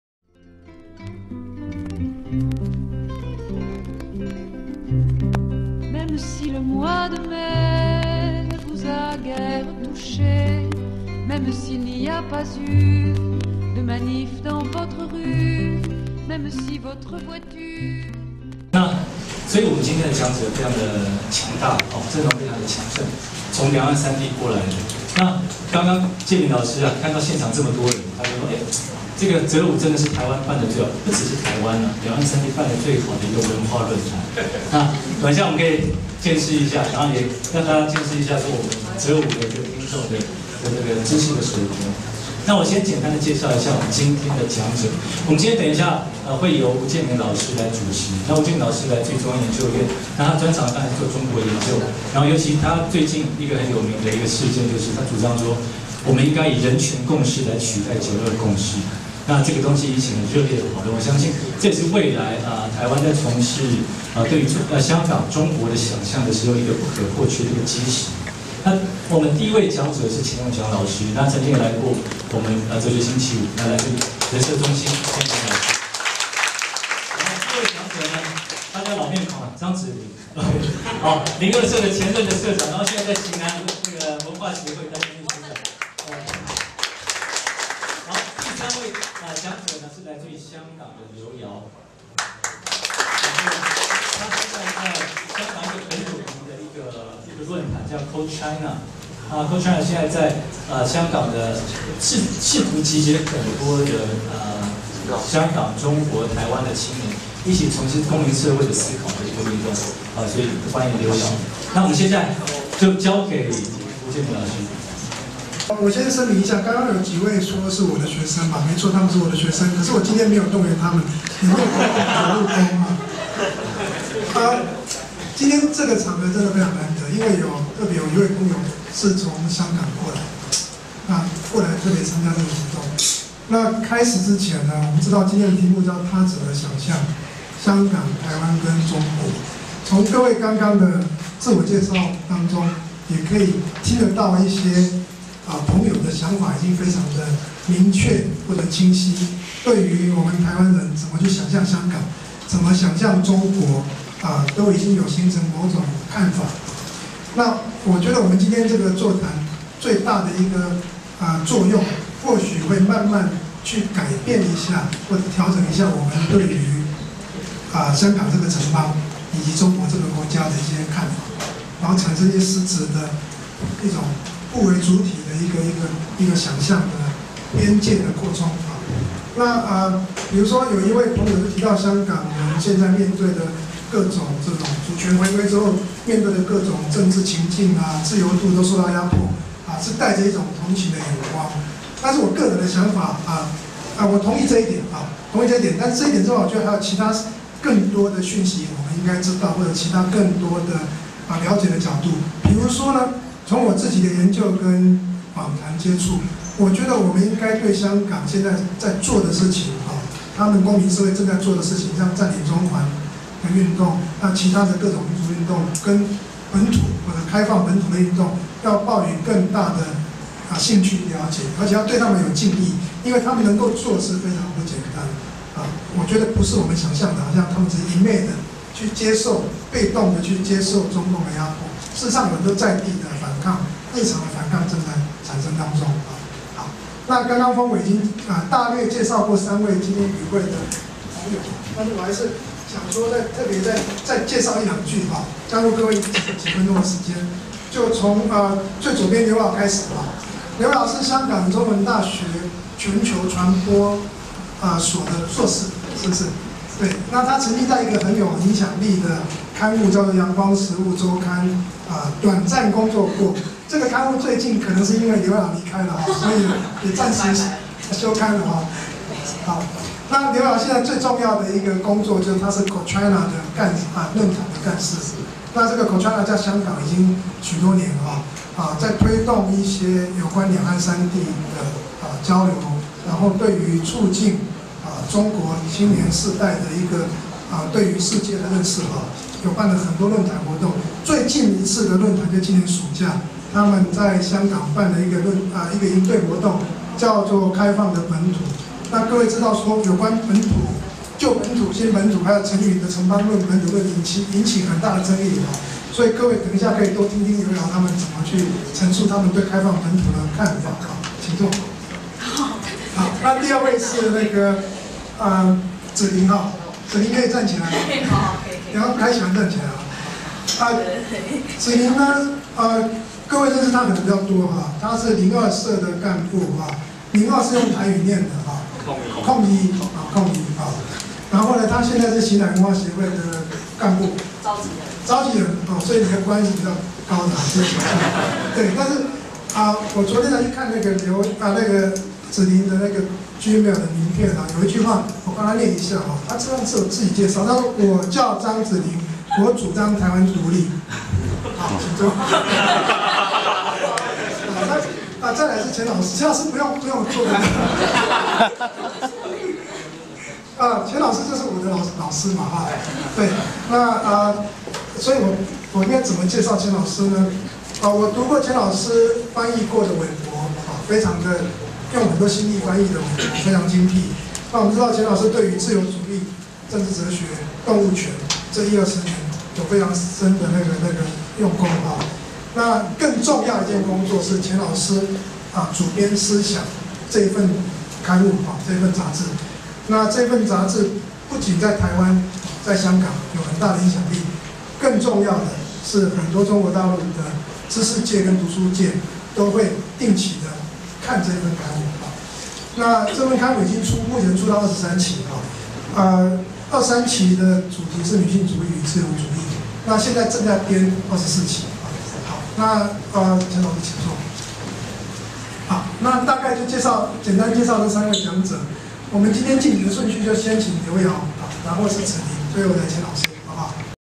【哲學星期五@台北】他者的想像─香港、台灣、中國 2013/08/30【開場】
【哲學星期五@台北】─「他者的想像─香港、台灣、中國」2013 / 08 / 30 (五) 19:30 @ Café Philo（慕哲咖啡館）

在長達一個世紀的歲月裡，台灣、香港、中國是彼此互相遙望的他者。三地有著截然不同的歷史發展軸線。香港被英國殖民百年，成為亞洲西化浪潮的先鋒、驕傲的東方明珠；台灣被日本殖民半個世紀，並且歷經半世紀之久的國民黨威權統治後，成為亞洲民主的看板；中國則九死一生、外患內亂不斷，在獨特的黨國一家「中國模式」下，以世界工廠之姿躋身世界第二大經濟體。

在彼此看似平行的發展軸線上，三者的關係卻千絲萬縷。在中國這個龐然大物之側，台灣和香港像是努力維持自轉的小行星，卻始終無法忽視中國的重力牽引。在香港，因文革風潮外溢而發生「六七暴動」，六四天安門事件之後促成民主派勢力日漸壯大，近來更因中國移民暴增而產生「蝗蟲論」；在台灣，國共內戰的冷戰思維形成政治論述的基本格局，中國崛起後，遊走兩岸的權貴更挾其龐大的資本，大有搖撼台灣政經體制之勢。

無論政治光譜的哪一側，都無法否認此一事實：台灣、香港、中國是三個無法互相忽視的他者。真正的問題是，我們應該如何「想像」彼此的關係？我們該如何認識這些看似熟悉、實則陌生的鄰居？在這一場論壇裡，讓我們擱置、拋棄過往官方的、教條的、由上而下灌輸的三地想像，讓我們從民間的、公民的身分開始彼此視域的交接。

本周【哲學星期五】很高興邀請到吳豐維與吳介民共同主持，並請到香港「我在中國」論壇(Co-China)的劉垚，與台灣的錢永祥、張芷菱，一起談談「他者」的經驗。

【時間】　2013年 08月 30日 (五) 19:30--21:30
【地點】　Café Philo 慕哲咖啡館 地下沙龍
【地址】　台北市紹興北街 3 號 B1
　　　　　(捷運板南線，善導寺站 6 號出口)
【主持】　吳豐維│美國紐約州立大學哲學博士
　　　　　吳介民│中央研究院社會學研究所副研究員
【與談】　劉　垚│「我在中國」（Co-China）論壇幹事
　　　　　錢永祥│中央研究院人社中心副研究員
　　　　　張芷菱│行南文化協會秘書長

【主辦】　哲學星期五志工團、青平台，慕哲社會企業
【協辦】　「我在中國」（Co-China）論壇

【策劃行政】廖健苡、陳廷豪、沈清楷【海報設計】徐清恬、楊郁婷【開場主持】廖品嵐【錄音】洪崇晏、丁宇徵【摘要】洪崇晏、凃京威【攝影】、梁家瑜、楊依陵、黃謙賢【逐字稿】蕭景文、吳政諭、江博緯【網管】黃昭華【行政協力】李明恩